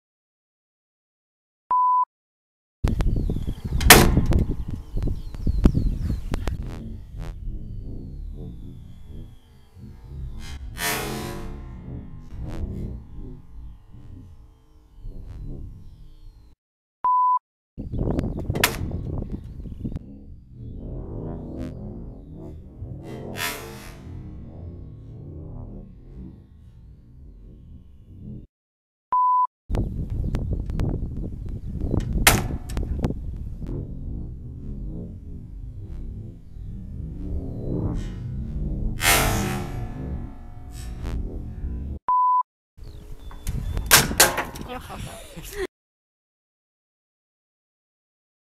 Testing 140# Turkish bow | Ivar Malde
Testing 140# @ 27,5" Turkish horn/sinew composite bow. Shot at 23,3" draw. With a 200 grain arrow, it gave 297 and 305 fps. The machine broke before testing at longer draw lenghts.